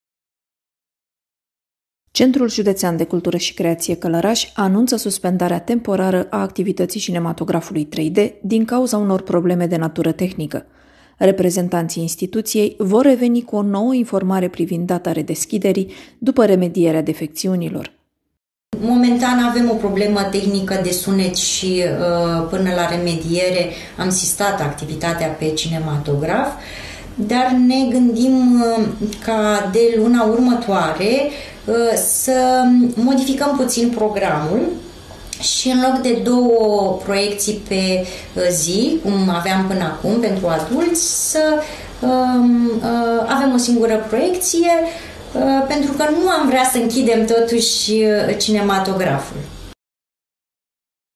Activitate suspendă la Cinematograful 3D
Se suspendă temporar activitatea Cinematografului 2D/3D, din cauza unor probleme de natură tehnică.